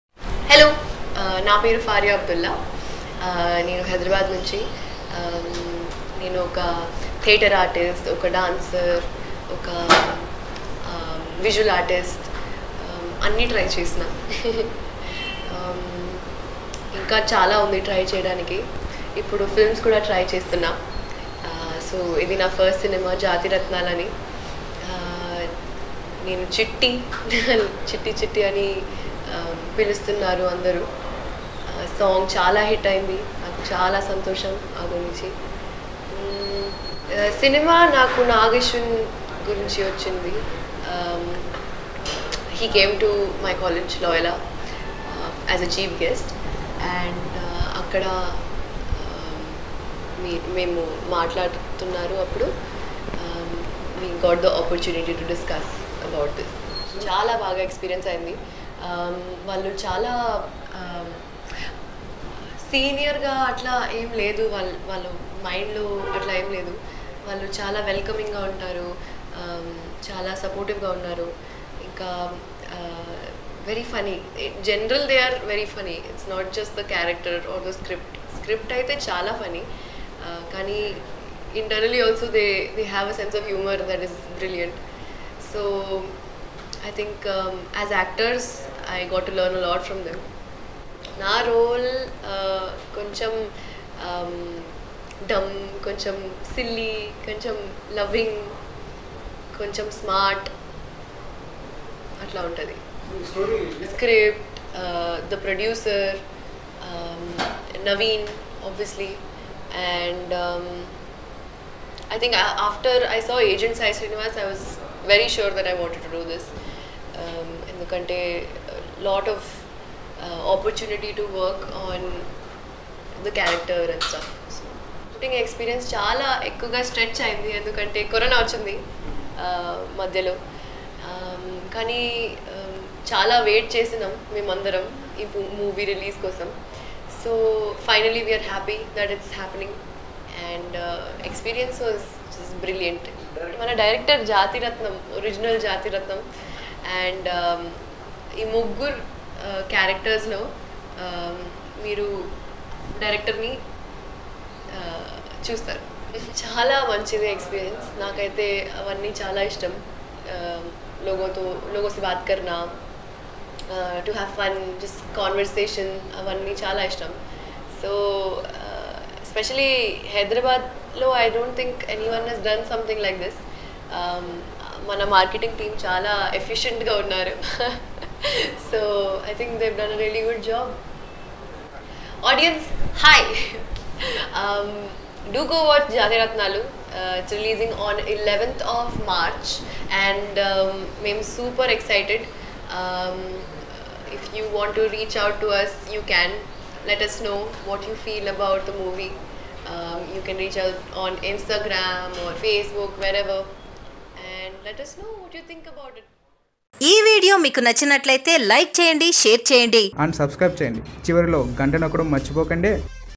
Actress Faria Abdullah Cute Words About Her Fans | Jathi Ratnalu | Naveen Polishetty | CC
Watch Here Actress Faria Abdullah Cute Words About Her Fans